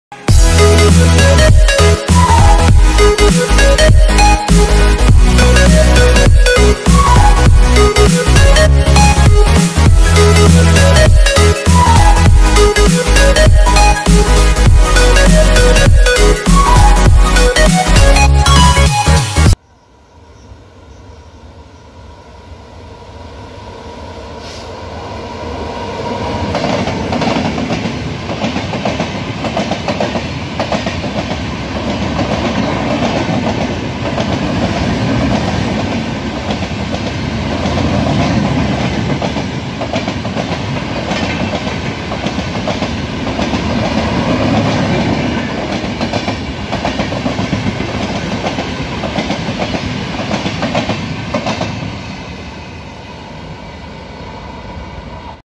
05959 Howrah-Dibrugarh Kamrup Special Skipping Balagarh and Maintained TSR
Location:-Balagarh(BGAE)

Loco:-HWH HOG WAP-7(#37182)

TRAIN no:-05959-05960

TRAIN name:-Kamrup Special